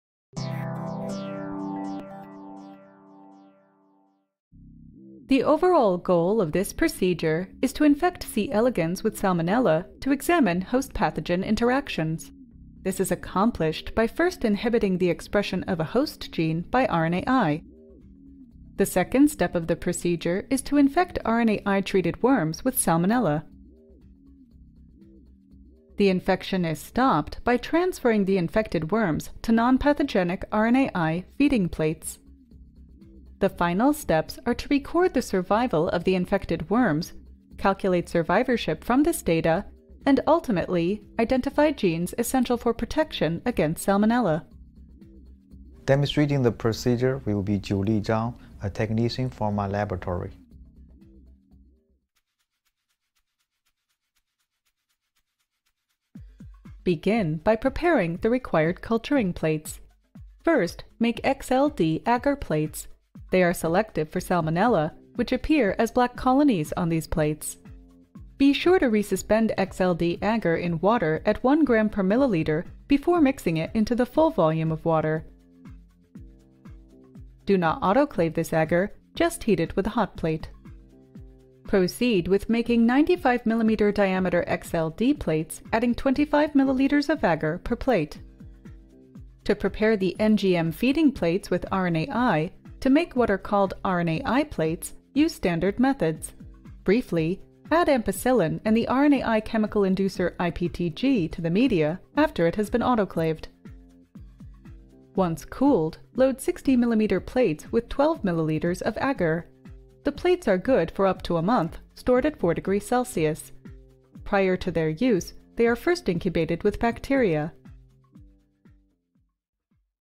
Infecting Free Living Nematodes with Pathogens| Protocol Preview
A Protocol to Infect Caenorhabditis elegans with Salmonella typhimurium -  a 2 minute Preview of the Experimental Protocol

Jiuli Zhang, Kailiang Jia
Florida Atlantic University, Department of Biological Sciences;

C. elegans has emerged as a new genetic model to study host-pathogen interactions. Here we describe a protocol to infect C. elegans with Salmonella typhimurium coupled with the double-strand RNAi interference technique to examine the role of host genes in defense against Salmonella infection.